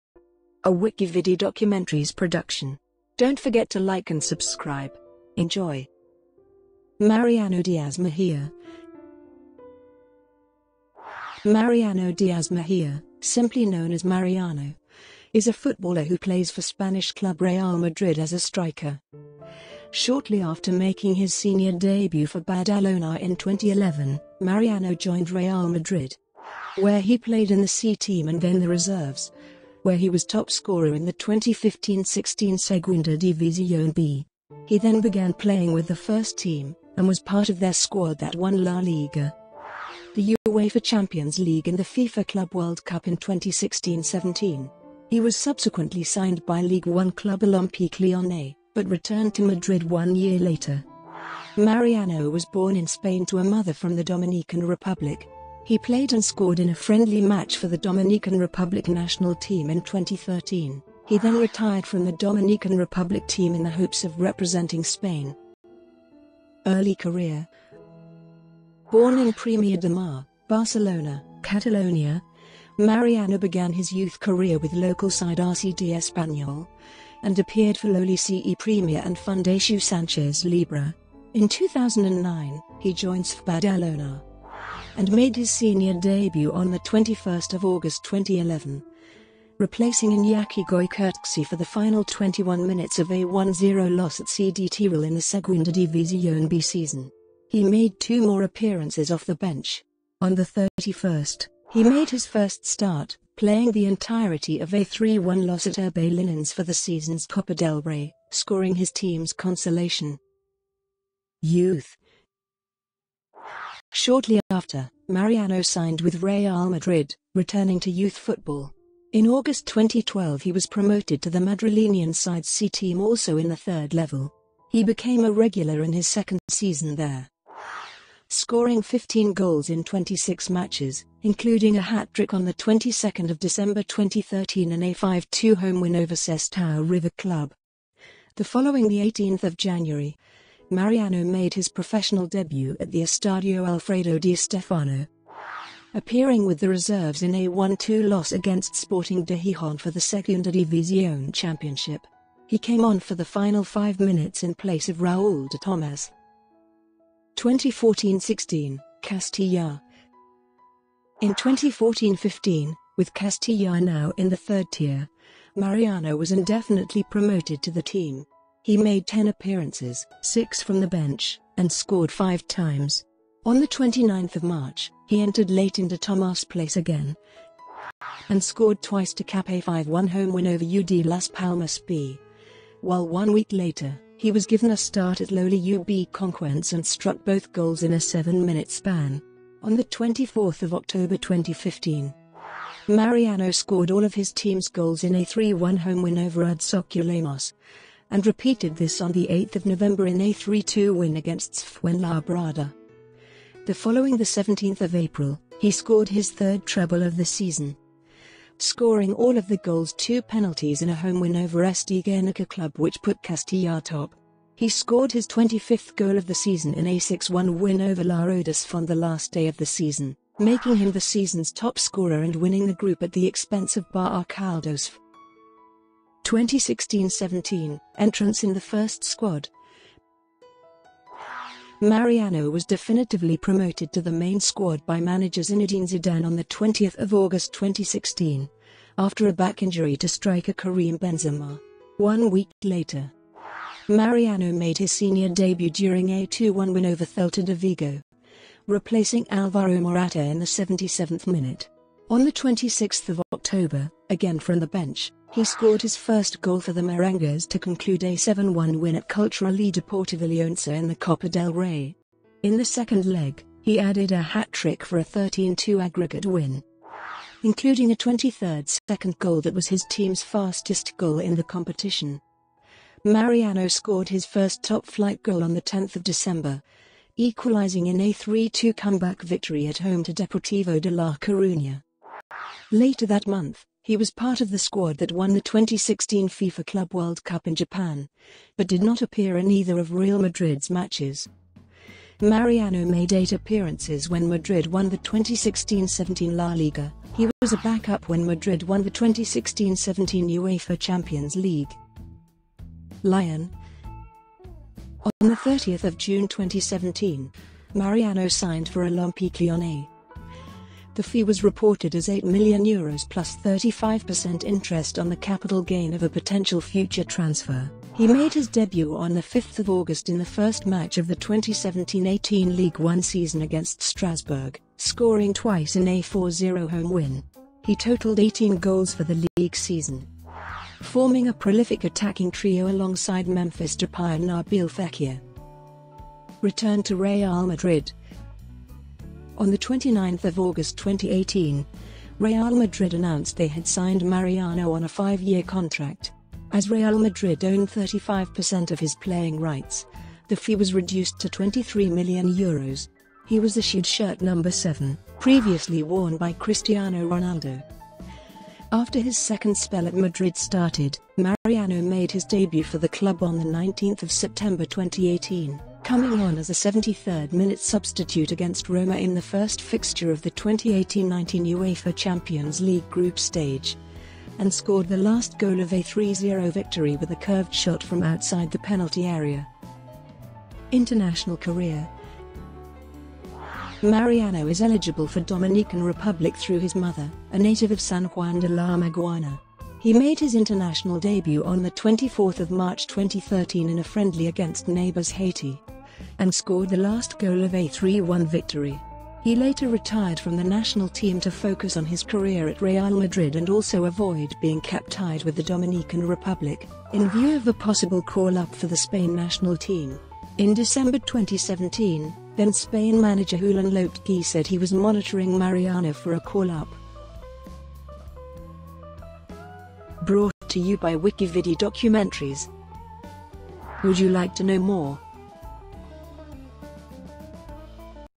MARIANO DÍAZ MEJÍA - WikiVidi Documentary
Mariano Díaz Mejía , simply known as Mariano, is a footballer who plays for Spanish club Real Madrid as a striker. Shortly after making his senior debut for Badalona in 2011, Mariano joined Real Madrid, where he played in the C-team and then the reserves, where he was top scorer in the 2015–16 Segunda División B. He then began playing with the first team, and was part of their squad that won La Liga, the UEFA Champions League and the FIFA Club World Cup in 2016–17. He was subsequently signed by Ligue 1 club Olympique Lyonnais, but returned to Madrid one year later. Mariano was born in Spain to a mother from the Dominican Republic. He played and scored in a friendly match for the Dominican Republic national team in 2013. He then retired from the Dominican Republic team in the hopes of representing Spain.

Shortcuts to chapters:
00:01:06 Early career
00:01:51 Youth
00:02:37 2014–16: Castilla
00:03:46 2016–17: Entrance in the first squad
00:05:02 Lyon
00:05:36 Return to Real Madrid

Copyright WikiVidi.